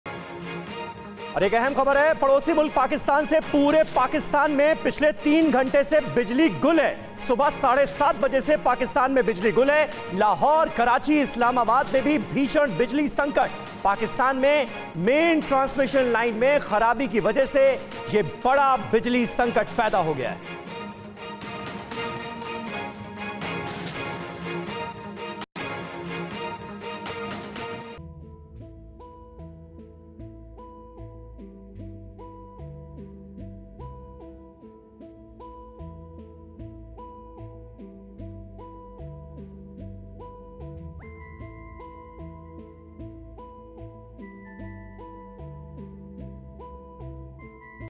Pakistan Power Cut: Pakistan की पिछले 3 घंटे से बिजली गुल, मचा हाहाकार
पाकिस्तान (Pakistan) की बिगड़ी आर्थिक स्थिति के बीच बिजली संकट (Pakistan Power Cut) गहरा गया है. राजधानी इस्लामाबाद के साथ-साथ लाहौर और कराची में घंटों से बिजली गुल है. पाकिस्तान में बीते कई दिनों से ऊर्जा संकट था. सरकार ने लोगों ने पावर सेविंग करने का अनुरोध भी किया था. यहां पर मॉल, बारात घर, मुख्य बाजार सभी को समय से पहले बंद करने का अनुरोध किया गया था. आज सुबह काफी देर तक लोगों ने लाइट का इंतजार किया लेकिन जब लाइट नहीं आई तो पता आसपास लोगों से पता किया.